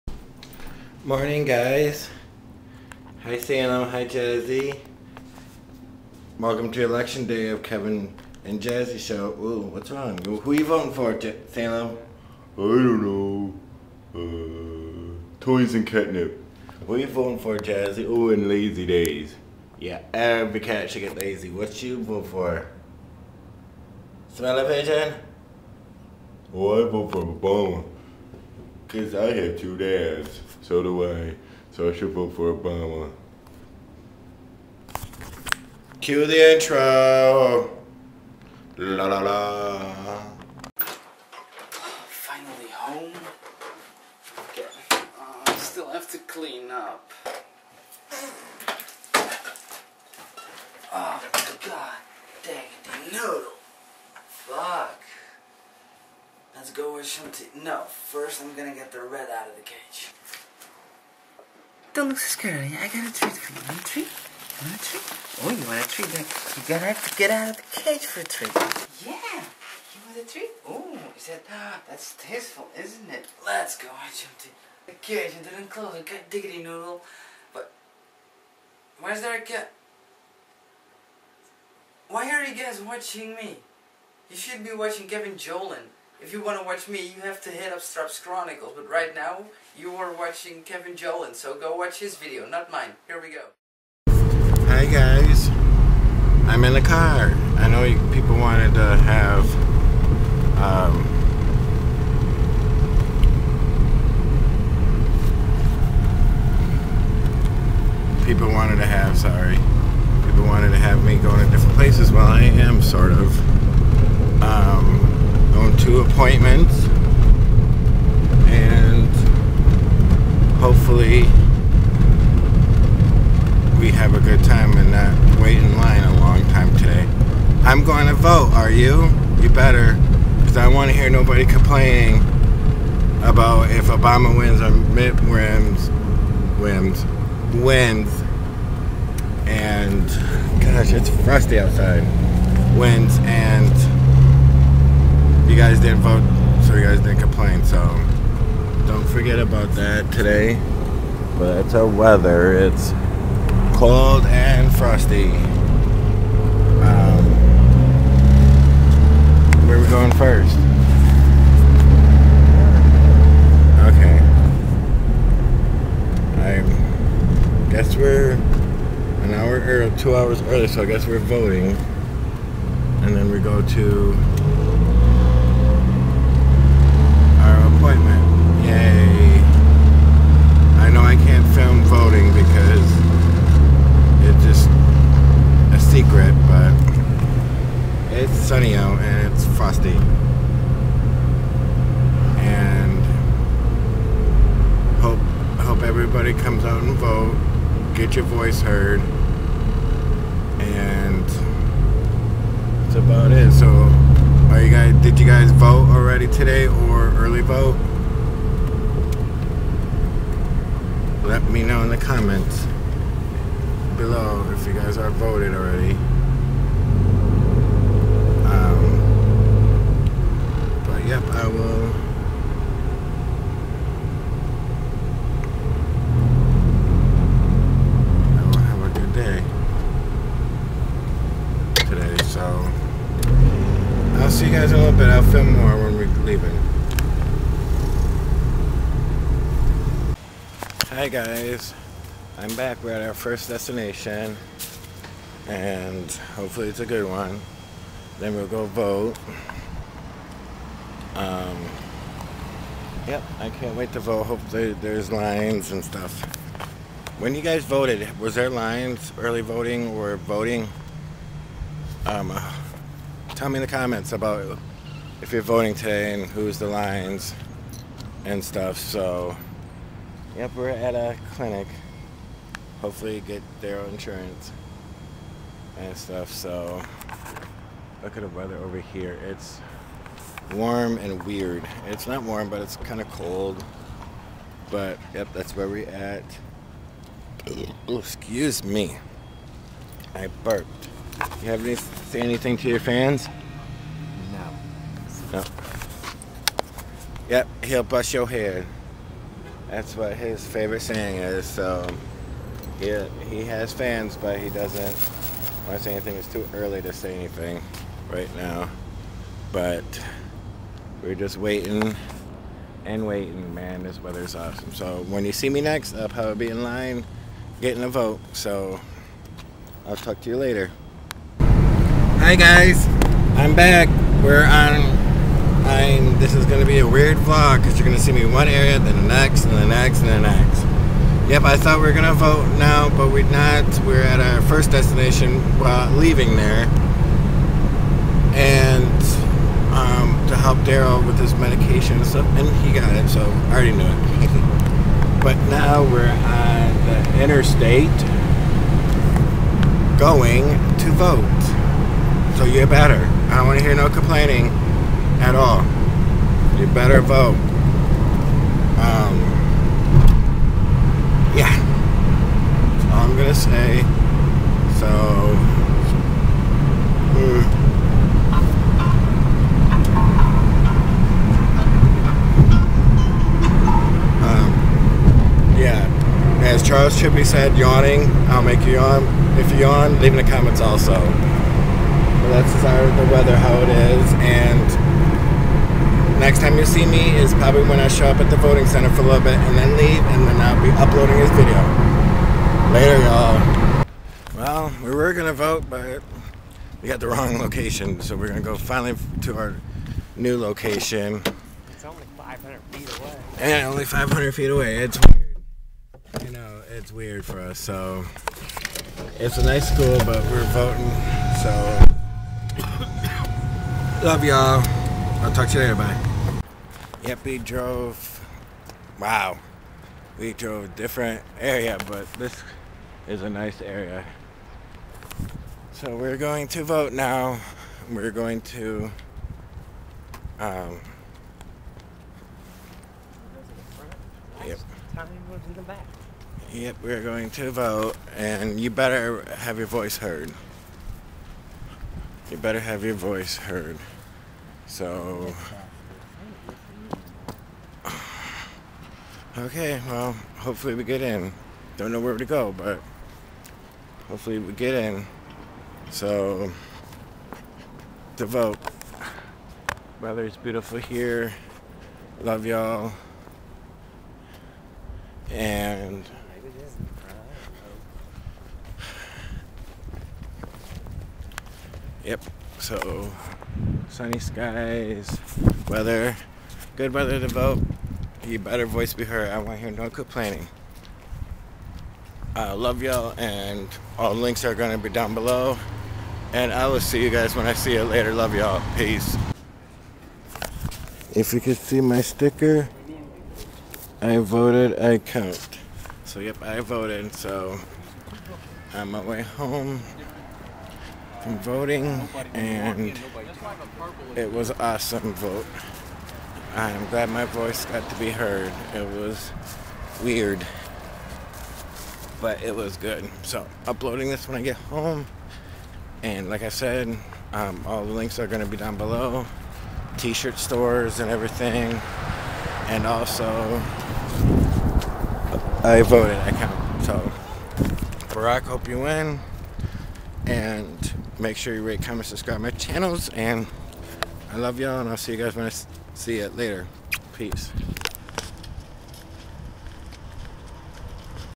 YTO I VOTED DID U #627
just running around and then voting i hope u vote today and let ur voice be heard.